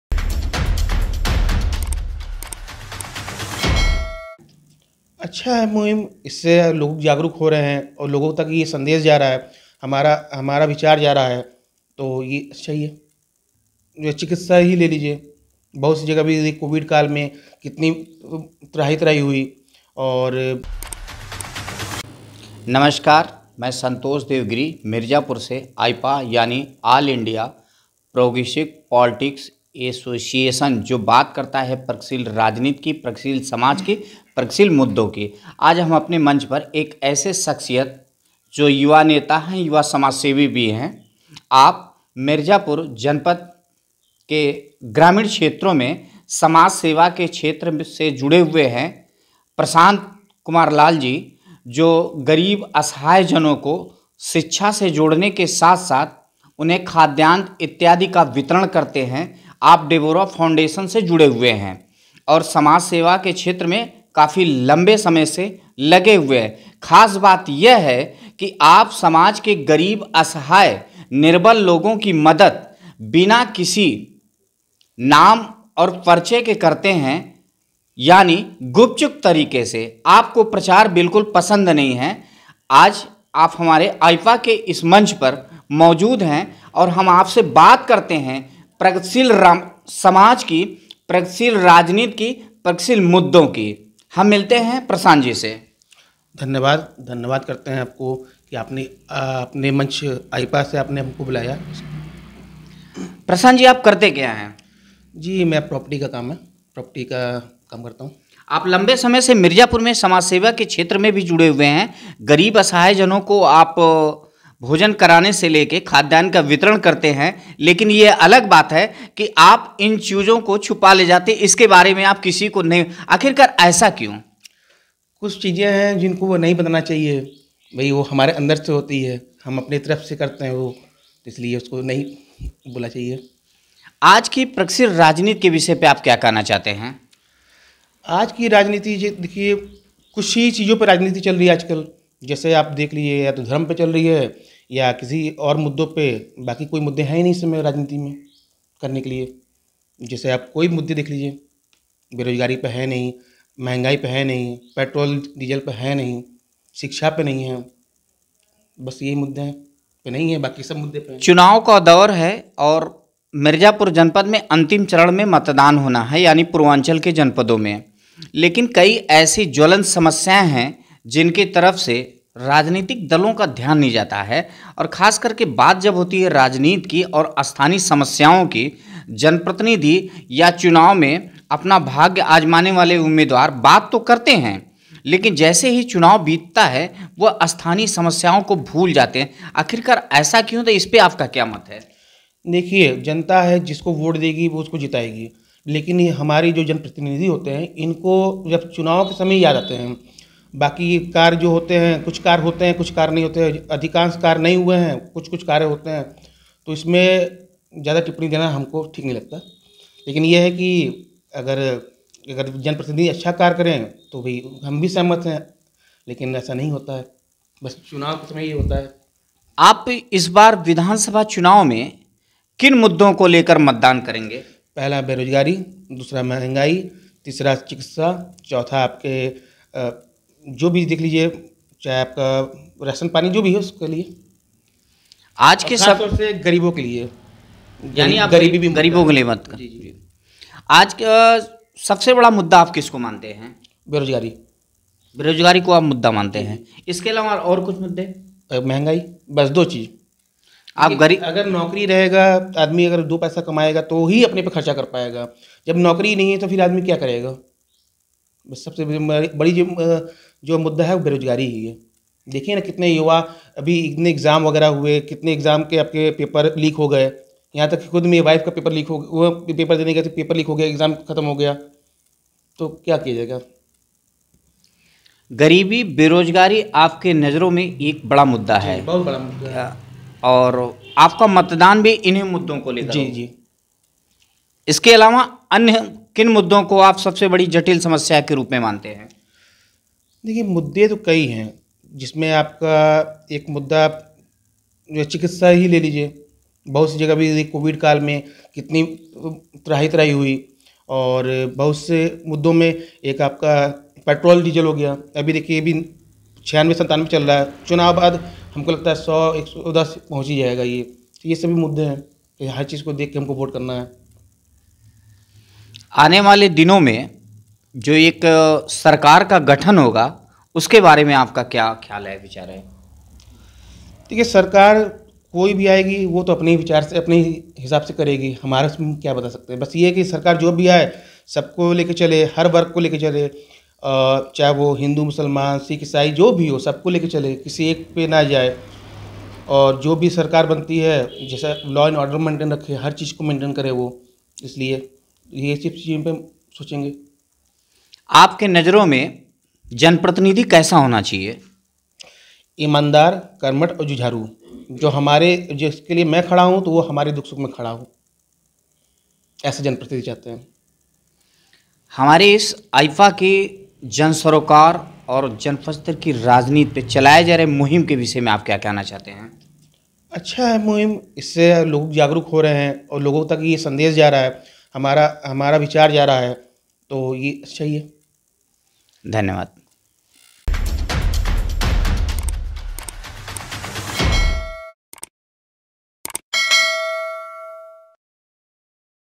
क्षेत्र का सबसे बड़ा मुद्दा है Poverty और Unemployment | AIPPA
जीतने के बाद कोई भी जनप्रतिनिधि क्षेत्र के विकास पर ध्यान नहीं देता है. ये बात AIPPA के मंच पर मिर्जापुर में समाजवादी पार्टी के पूर्व नगर अध्यक्ष प्रशांत कुमार लाल ने कही. उनसे AIPPA सहयोगी संतोष देव गिरी ने बातचीत की. उन्होंने और क्या कहा, जानने के लिए देखिए, ये वीडियो -  क्षेत्र का सबसे बड़ा मुद्दा है Poverty और Unemployment

Produced By: All India Progressive Politics Association